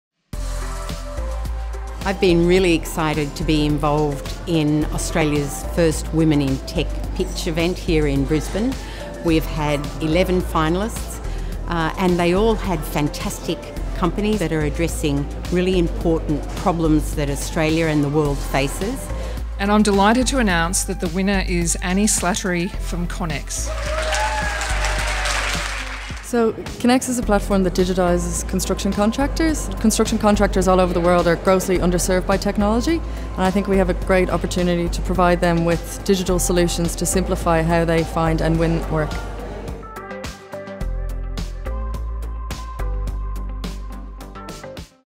Australian Women in Tech Pitch 2019
Australian Women in Tech Pitch Event is brought to you by the Australian Government - through the Australian Trade and Investment Commission (Austrade), Department of Industry, Innovation and Science (AusIndustry) and CSIRO/Data61.